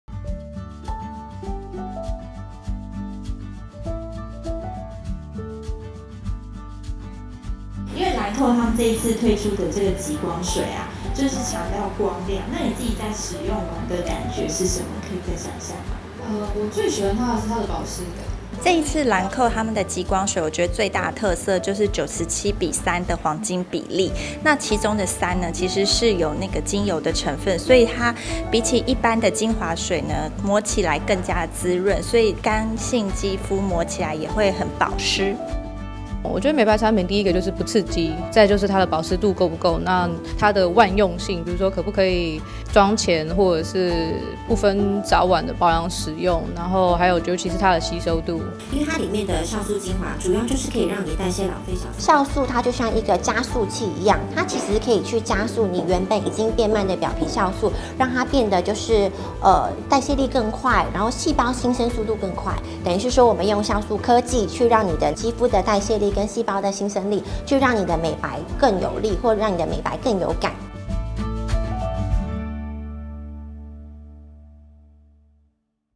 【VOGUE靚技場× Lancome】這個世代女生最嚮往的光澤肌養成秘訣
身為資深保養控不可以不知道，這個從天地洪荒時就已經存在、能讓肌膚維持透亮的關鍵成分！
快來偷聽 超夯美妝部落客 少女人妻Alice和美容編輯Jasmine大聊的美白保養經！